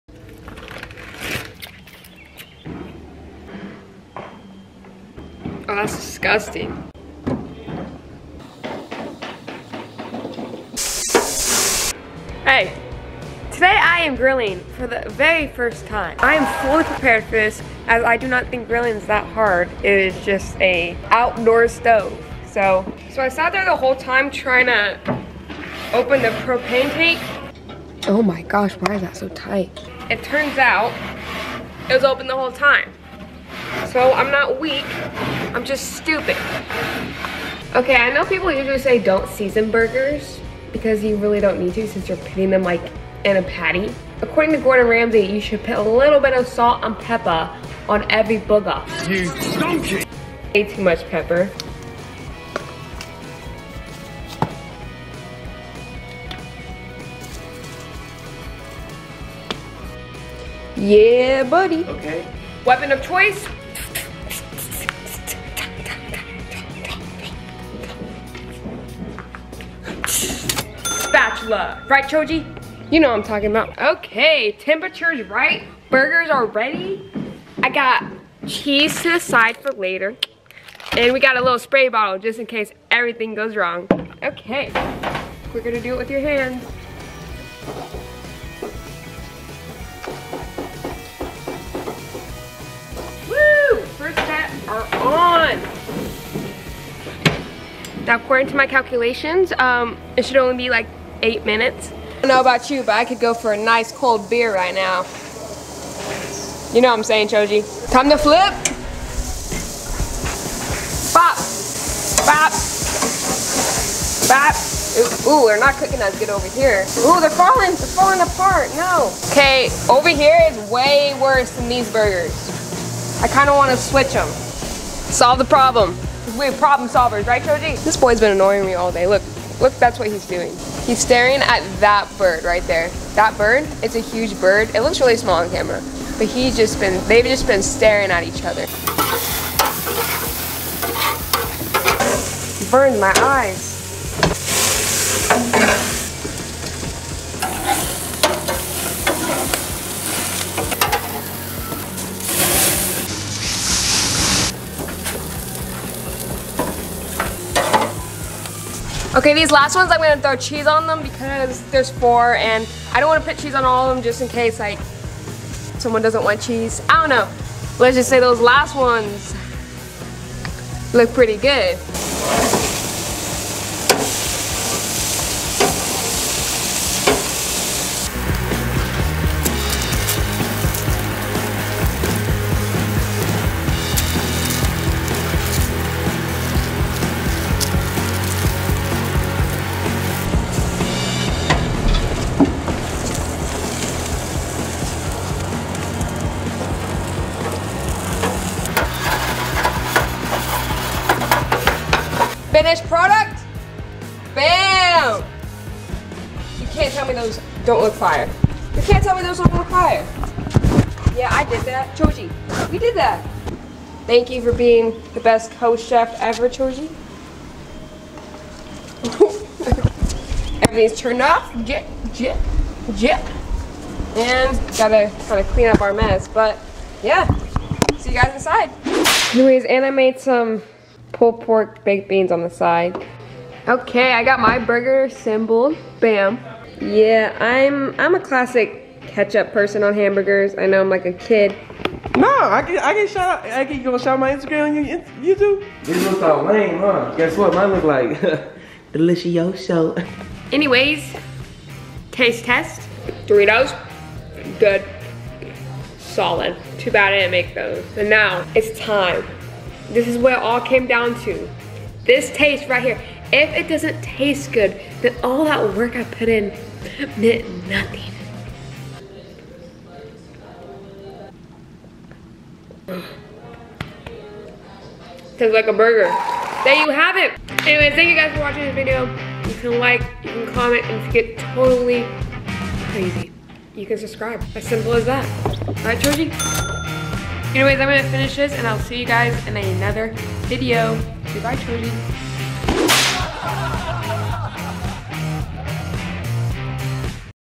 Teaching MYSELF how to GRILL!!!
Welcome to me mastering the ART of Grilling on the first Try. yea...um so i basically just teach my self how to grill in this video do i burn my eyebrows off? I don't know i guess there is one way to find out! Thank yAll for watching if u did! Ill see yall in another description!!!